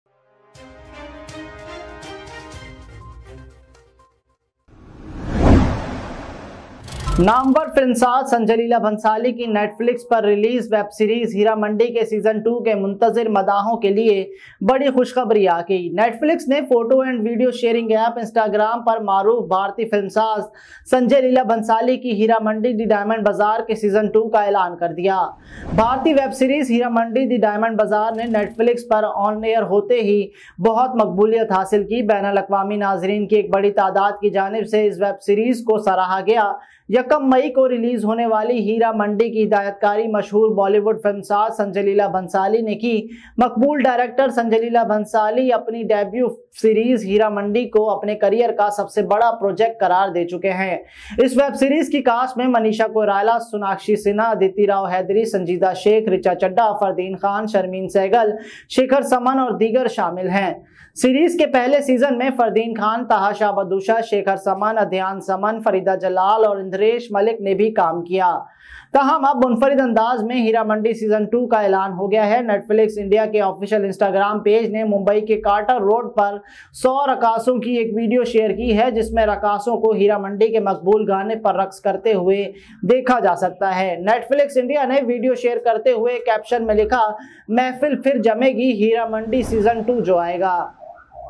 Sanjay Leela Bhansali Hit Heeramandi The Diamond Bazaar; Season 2 on Netflix | Wahjoc Entertainment
Sanjay Leela Bhansali Hit ‘Heeramandi: The Diamond Bazaar’ Renewed for Season 2 by Netflix | Wahjoc Entertainment